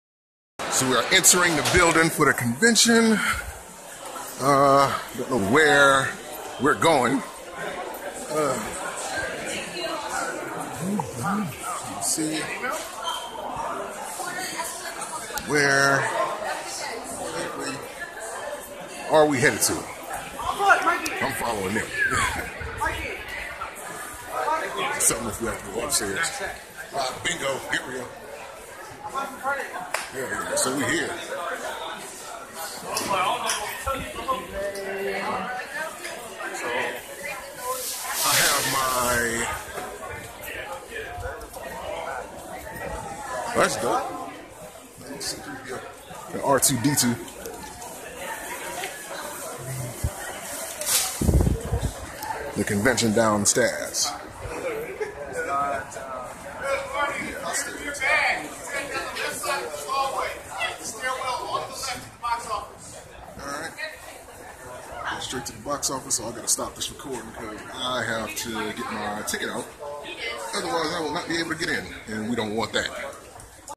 San Francisco Comic-con 9/13/16 pt4 Into the Merriot
This is my footage from my trip to the San Francisco comic-con. I wasnt able to get much. 1: My leg was hurting and I had to rest often. 2: I had no clue their was 6 floors to the comic-con & 3: my phone battery died. But I got what I could.